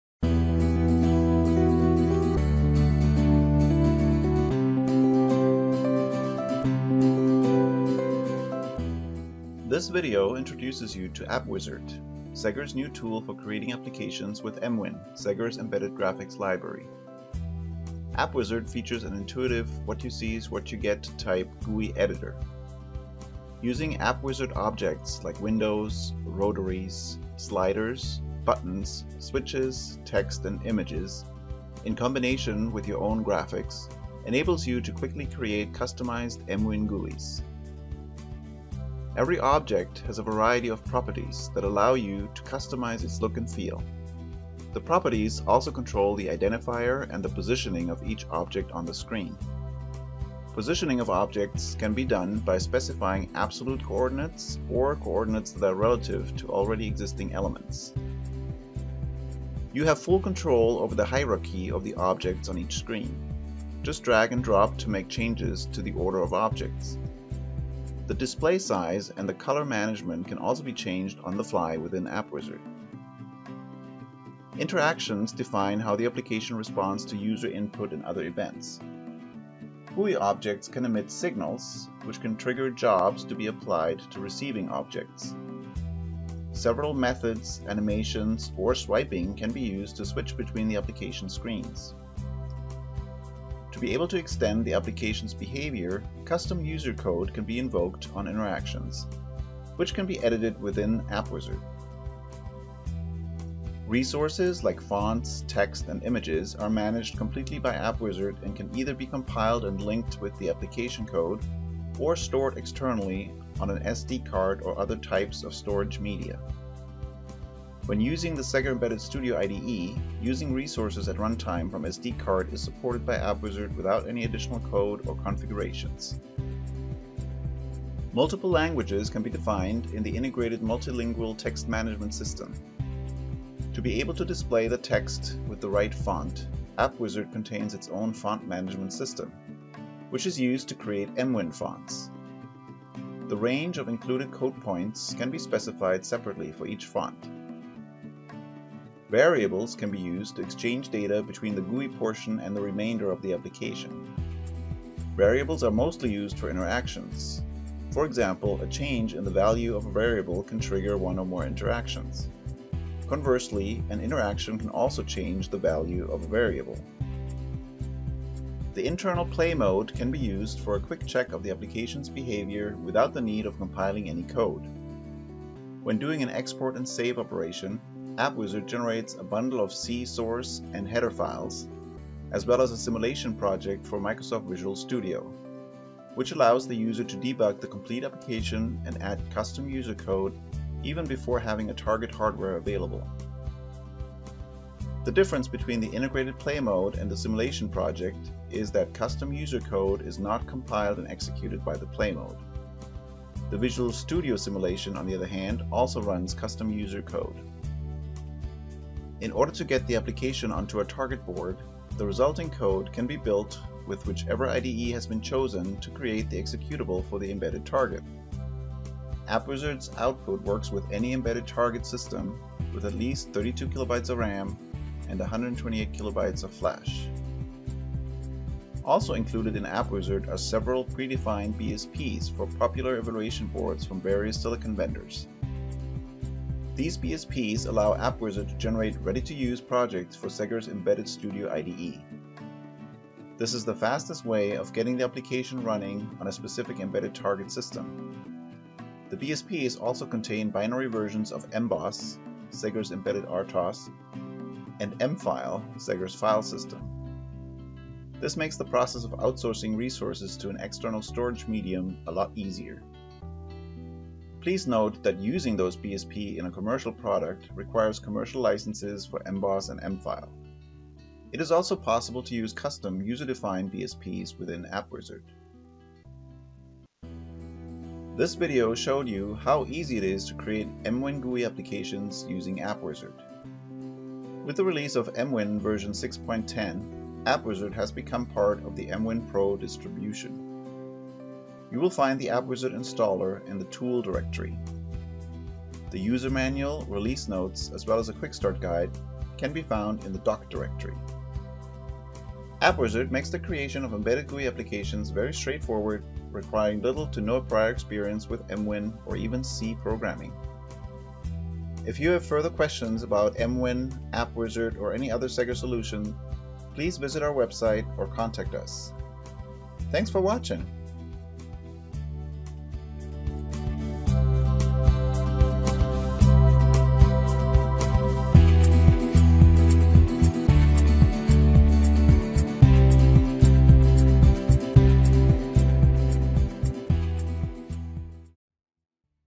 SEGGER emWin AppWizard — Introduction
This video introduces you to AppWizard, SEGGER's new GUI design tool for the SEGGER emWin embedded graphics library.

What is AppWizard?

AppWizard is a powerful tool for creating complete and ready-to-run applications. With its many features, AppWizard makes it amazingly easy and simple to create even complex graphical applications without requiring in-depth knowledge of the emWin graphics library or even the C language for coding.